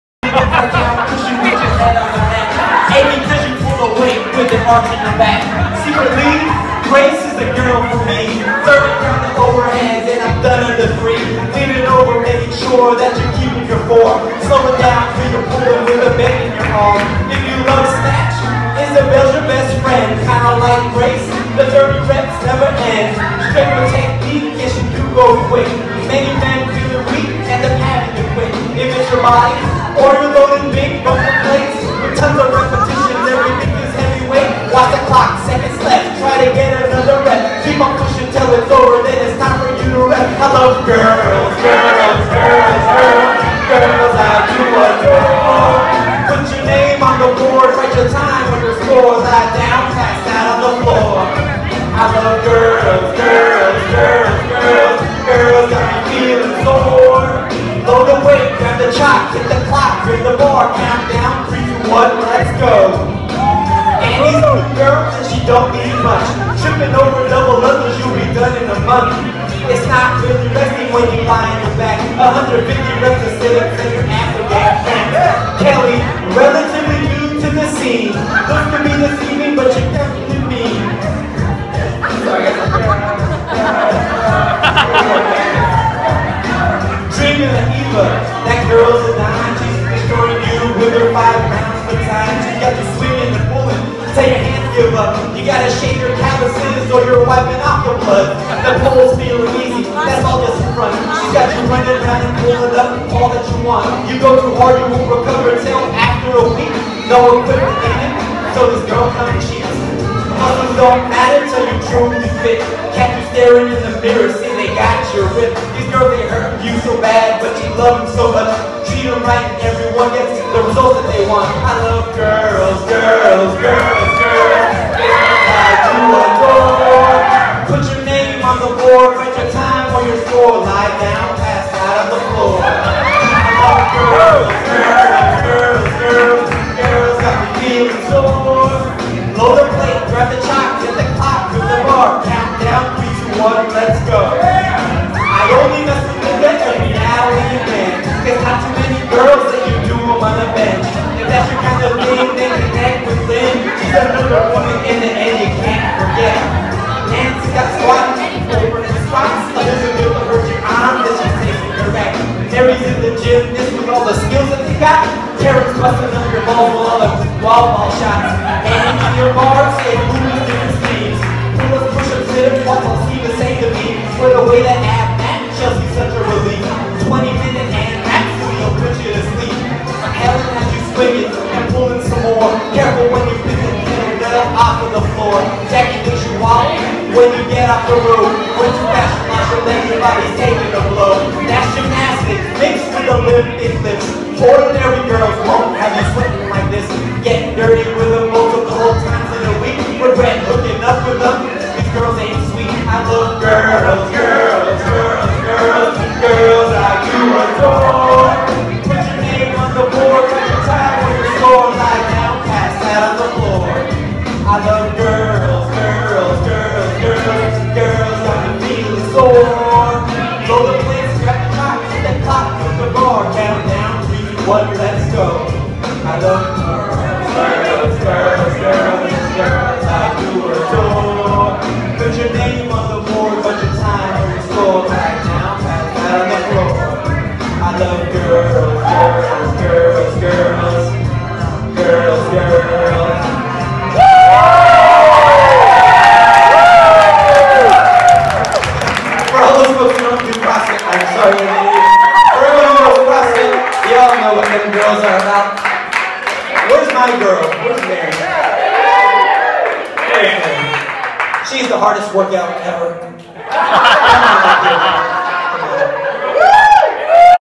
Chris Junsay's CrossFit Rap Song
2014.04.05
CJ performing his CrossFit rap song at his 33rd Birthday party.